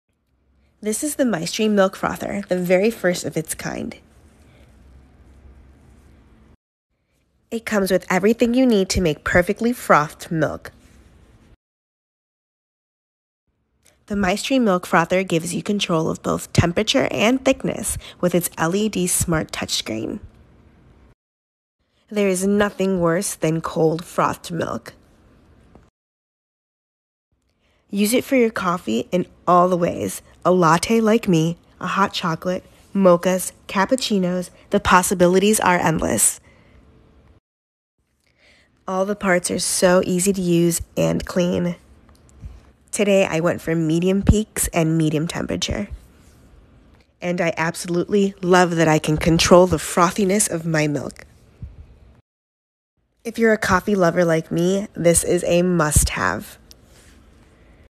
I Tested Best Milk Frother 2022 For Home Baristas
For a home Baristas, this frother definitely does a great job! Finding a milk frother is very easy, however, finding a milk frother that is high-quality can be a bit more difficult. You might as well try the Maestri House milk frother.

""Things I love about the Maestri House Milk Frother:
•temperature & thickness control
•led touchscreen
•hands-free!
•cute design
•super easy to clean
•multiple uses

It’s so easy to use and clean and perfect for multiple cups of coffee. Love that I don’t have to stand there with a handheld frother anymore! " said @
elzamante.

Here are some Coffee Recipes from Maestri House. The frothy coffee is fantastic and can be enjoyed at any time of the day.

Follow us to get more latest news!